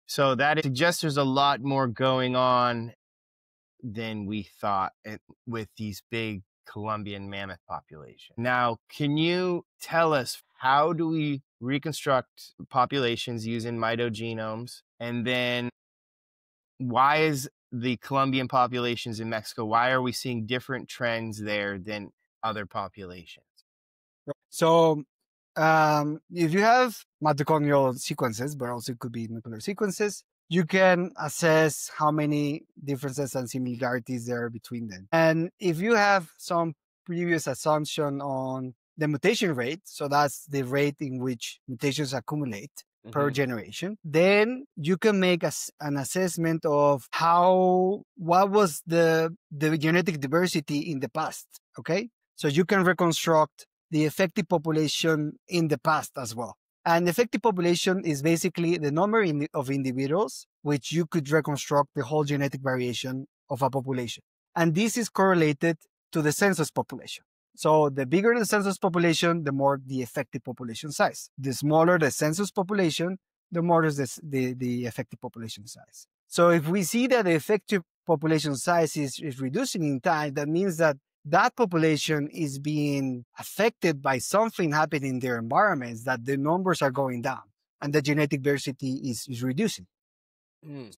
How mtDNA Reconstructs Mammoth Population Size
How can DNA act like a time machine? In this clip, Dr. Federico Sánchez-Quinto and I discuss using mitochondrial genomes to reconstruct changes in mammoth population size and diversity—revealing when numbers surged or sank and why it matters for Ice Age ecology.